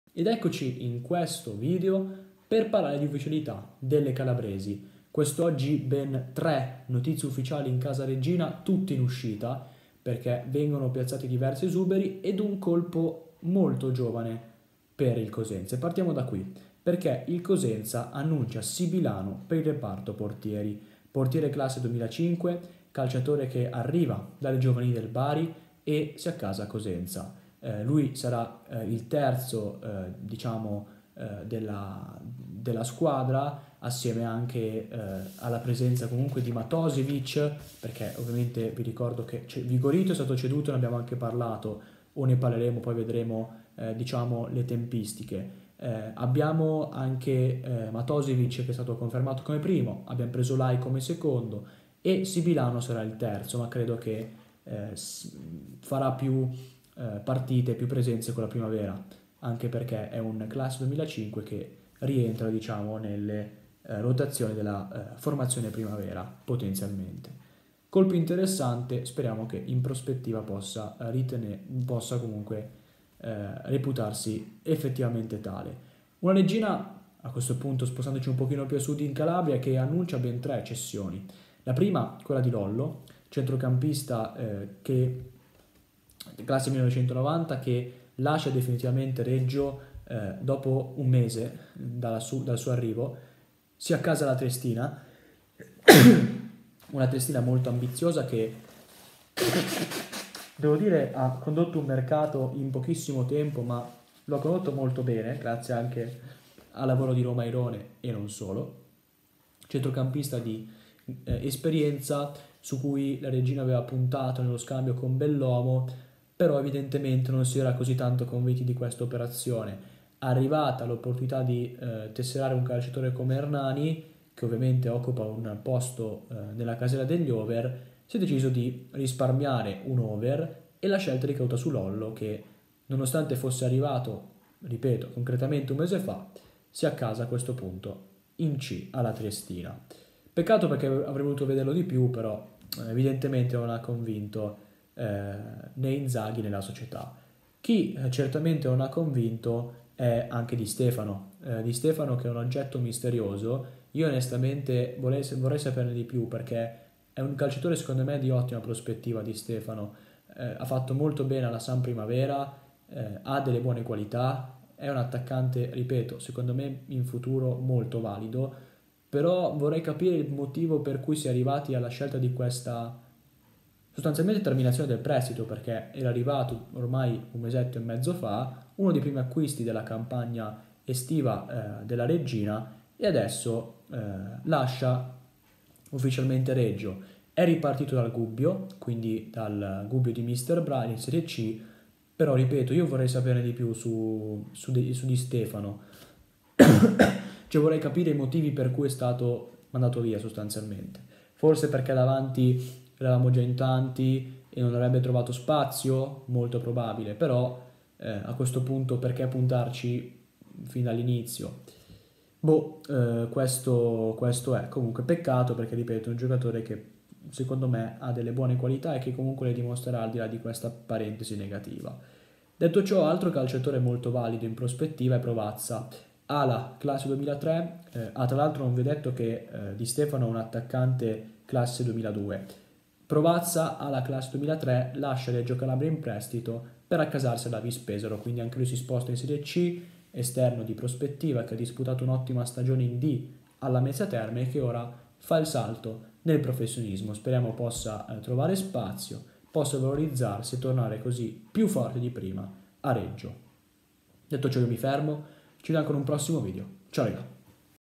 SIBILANO AL COSENZA, LOLLO ALLA TRIESTINA, GAVIOLI ALLA PRO PATRIA E DI STEFANO LASCIA LA REGGINA
In questo video andiamo a parlare dell'arrivo di Sibilano al Cosenza, delle cessioni, da parte della Reggina, di Lollo alla Triestina e di Gavioli alla Pro Patria. In più andiamo a parlare del fatto che Di Stefano lascia la Reggina